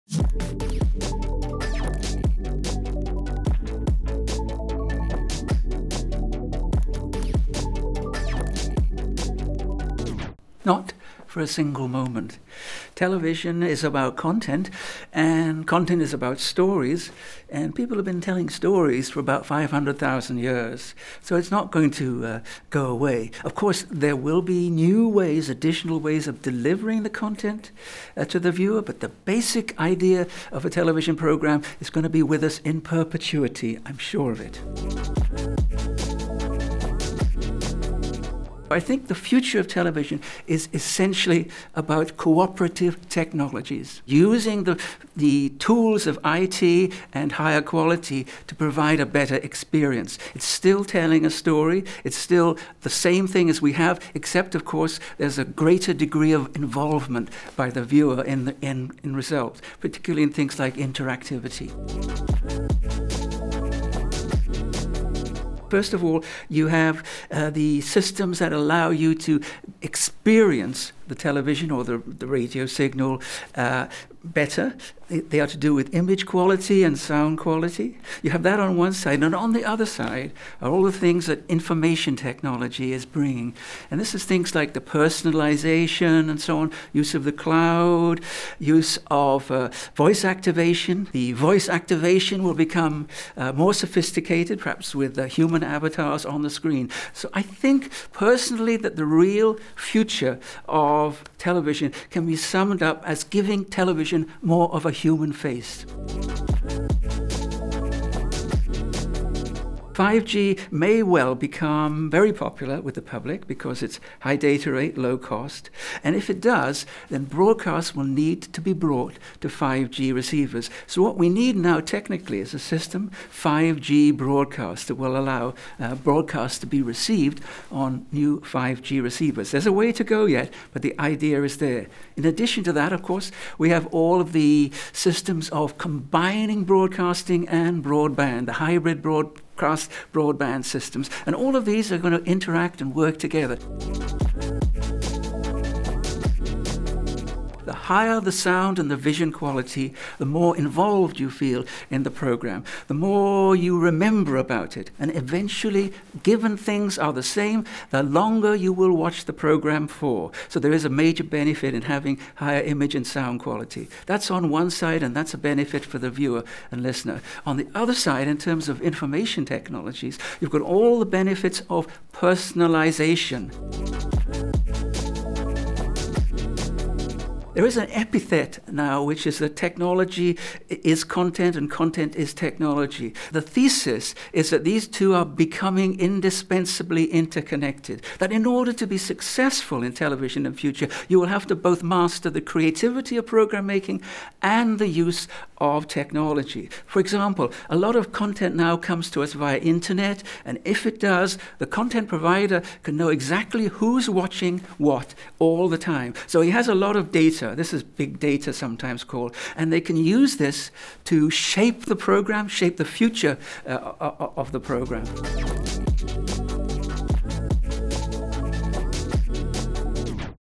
World TV Day: A Look at the Future of Television (International Version - no graphics or captions)
Available as download for Media, as well as English version (captioned).

Is television obsolete? How will new technologies shape TV? What about 5G and TV?  David Wood, Consultant Technology and Innovation, European Broadcasting Union (EBU) and Member ITU-R Study Group 6, gives us his insights into the future of television for World Television Day 2019, 21 November 2019.

A UNTV, ITU, EBU production.